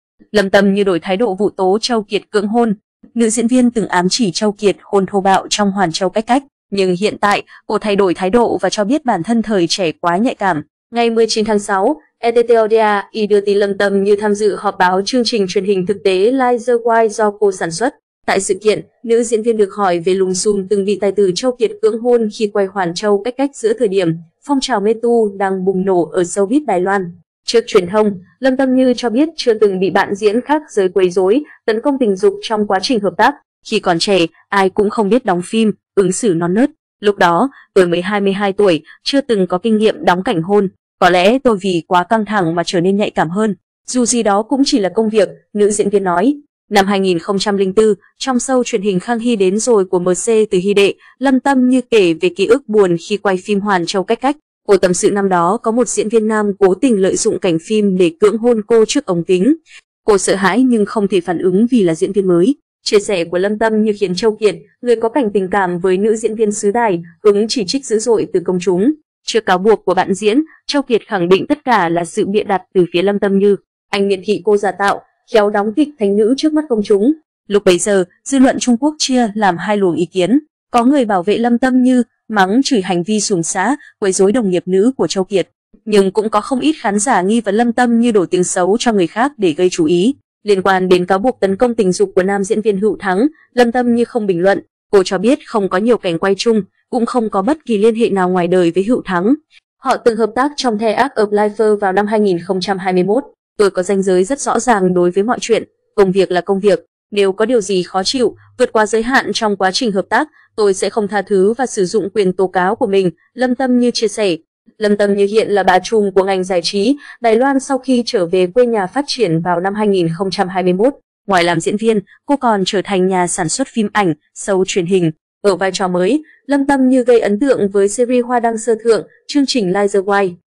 Lâm Tâm Như đổi thái độ vụ tố Châu Kiệt cưỡng hôn
Nữ diễn viên từng ám chỉ Châu Kiệt hôn thô bạo trong 'Hoàn Châu cách cách'. Nhưng hiện tại, cô thay đổi thái độ và cho biết bản thân thời trẻ quá nhạy cảm."Tôi có ranh giới rất rõ ràng đối với mọi chuyện. Công việc là công việc. Nếu có điều gì khó chịu, vượt quá giới hạn trong quá trình hợp tác, tôi sẽ không tha thứ và sử dụng quyền tố cáo của mình", Lâm Tâm Như chia sẻ.
Lâm Tâm Như hiện là "bà trùm" của ngành giải trí Đài Loan sau khi trở về quê nhà phát triển vào năm 2021. Ngoài làm diễn viên, cô còn trở thành nhà sản xuất phim ảnh, show truyền hình. Ở vai trò mới, Lâm Tâm Như gây ấn tượng với series Hoa đăng sơ thượng, chương trình Light The Wild.
Lâm Tâm Như - Châu Kiệt cắt đứt quan hệ sau cáo buộc bị bạn diễn cưỡng hôn từ phía người đẹp xứ Đài. Ảnh: Sina.
Liên quan đến cáo buộc tấn công tình dục của nam diễn viên Hựu Thắng, Lâm Tâm Như không bình luận. Họ từng hợp tác trong The Arc of Life vào năm 2021.
Trước truyền thông, Lâm Tâm Như cho biết chưa từng bị bạn diễn khác giới quấy rối, tấn công tình dục trong quá trình hợp tác. "Khi còn trẻ, ai cũng không biết đóng phim, ứng xử non nớt. Lúc đó, tôi mới 22 tuổi, chưa từng có kinh nghiệm đóng cảnh hôn. Có lẽ tôi vì quá căng thẳng mà trở nên nhạy cảm hơn. Dù gì đó cũng chỉ là công việc", nữ diễn viên nói.
Chia sẻ của Lâm Tâm Như khiến Châu Kiệt - người có cảnh tình cảm với nữ diễn viên xứ Đài - hứng chỉ trích dữ dội từ công chúng. Trước cáo buộc của bạn diễn, Châu Kiệt khẳng định tất cả là sự bịa đặt từ phía Lâm Tâm Như. Anh miệt thị cô giả tạo, khéo đóng kịch "thánh nữ" trước mắt công chúng.
Năm 2004, trong show truyền hình Khang Hy đến rồi của MC Từ Hy Đệ, Lâm Tâm Như kể về ký ức buồn khi quay phim Hoàn Châu cách cách. Cô tâm sự năm đó có một diễn viên nam cố tình lợi dụng cảnh phim để cưỡng hôn cô trước ống kính. Cô sợ hãi nhưng không thể phản ứng vì là diễn viên mới.
Lúc bấy giờ, dư luận Trung Quốc chia làm 2 luồng ý kiến. Có người bảo vệ Lâm Tâm Như, mắng chửi hành vi suồng sã, quấy rối đồng nghiệp nữ của Châu Kiệt. Nhưng cũng có không ít khán giả nghi vấn Lâm Tâm Như đổ tiếng xấu cho người khác để gây chú ý.
Ngày 19/6, ETtoday đưa tin Lâm Tâm Như tham dự họp báo chương trình truyền hình thực tế Light The Wild do cô sản xuất. Tại sự kiện, nữ diễn viên được hỏi về lùm xùm từng bị tài tử Châu Kiệt cưỡng hôn khi quay Hoàn Châu cách cách giữa thời điểm phong trào Metoo đang bùng nổ ở showbiz Đài Loan.